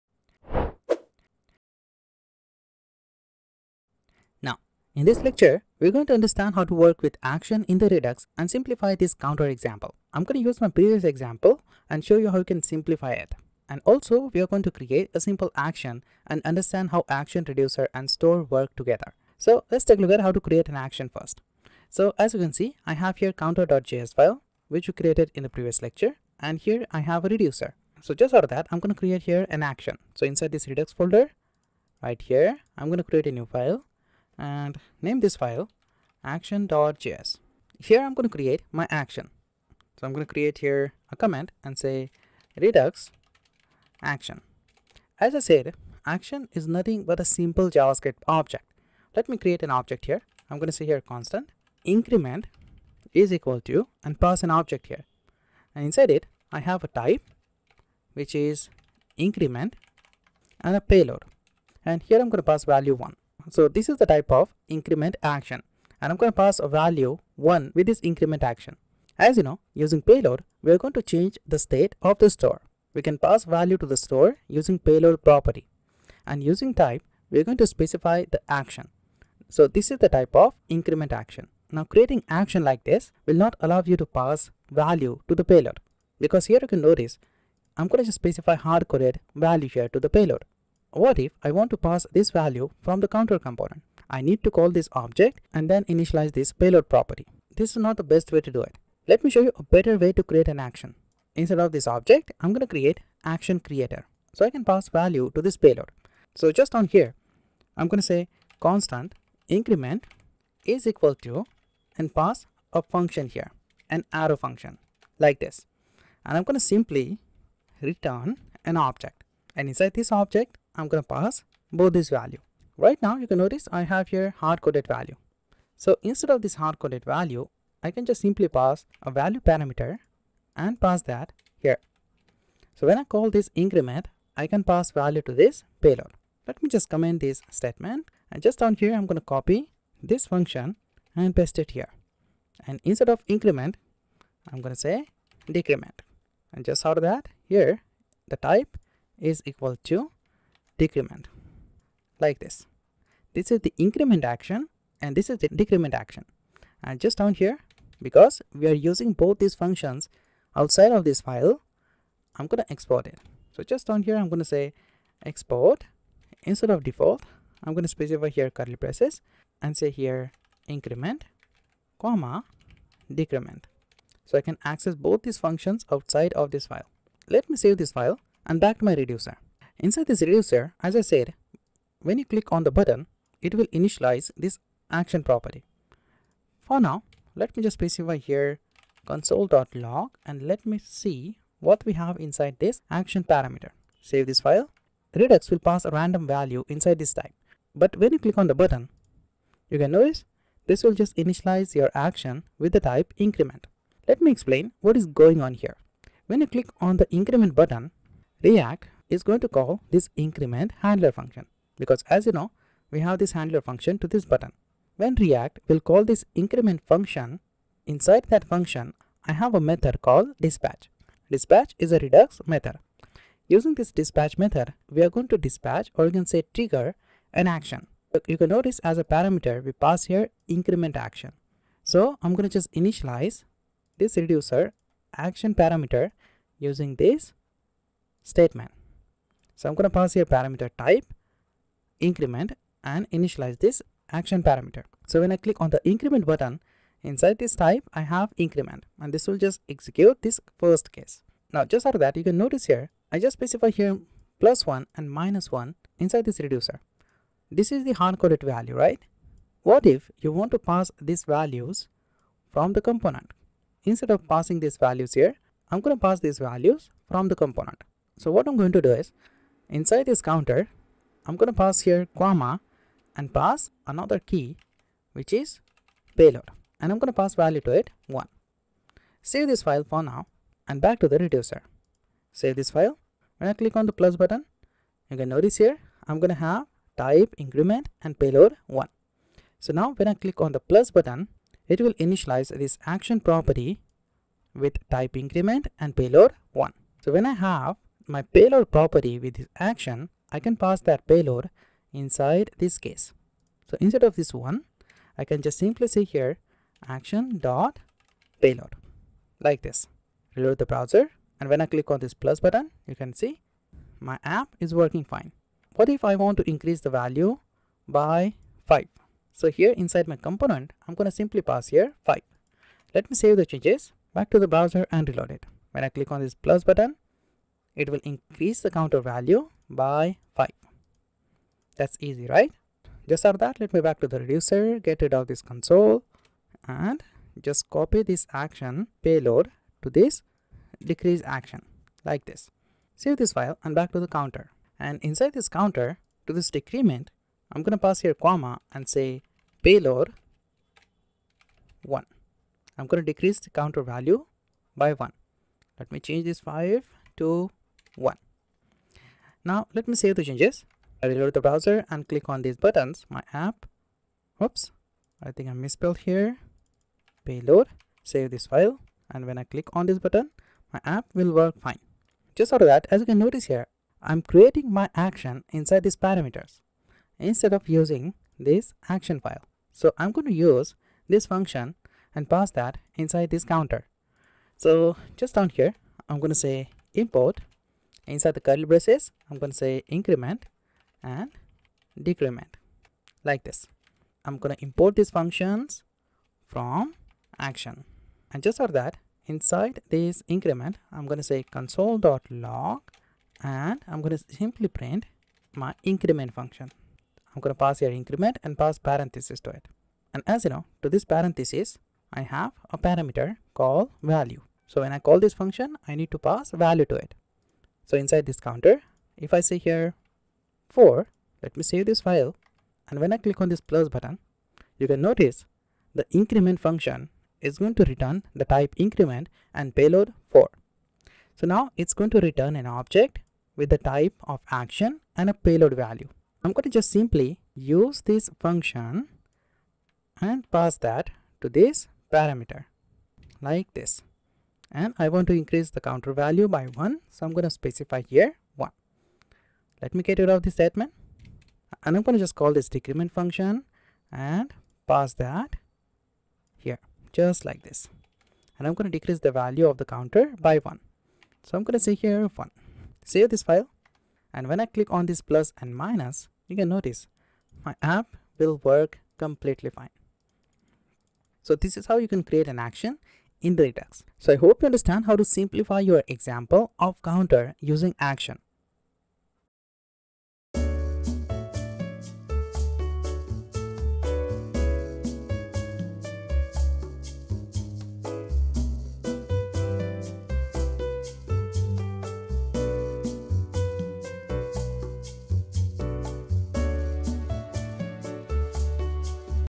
Redux Action Creator - MERN Stack Series - 50
In this lecture, we are going to learn how to create an action in the redux. we all know how to create a counter example. We are going to update that example and the action inside it.